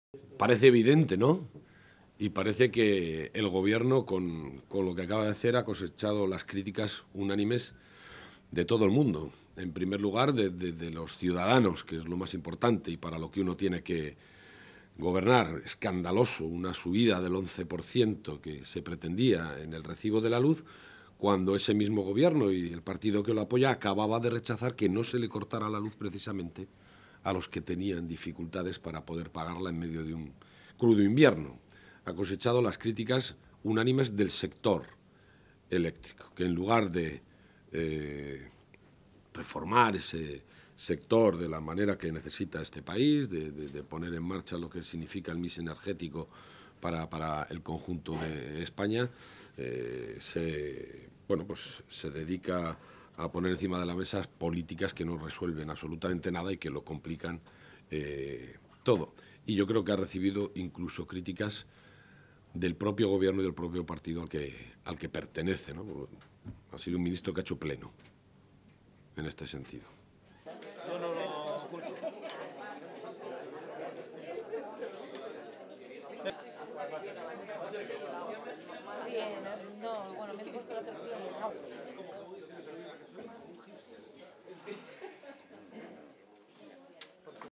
López: "Soria ha provocado críticas unánimes"
El secretario general del PSE-EE, Patxi López, ha denunciado que las políticas en materia energética del Gobierno central "no resuelven nada y lo complican todo" y, por ello, el ministro de Industria, José Manuel Soria, "ha hecho pleno" al provocar "críticas unánimes" de los ciudadanos y del sector eléctrico.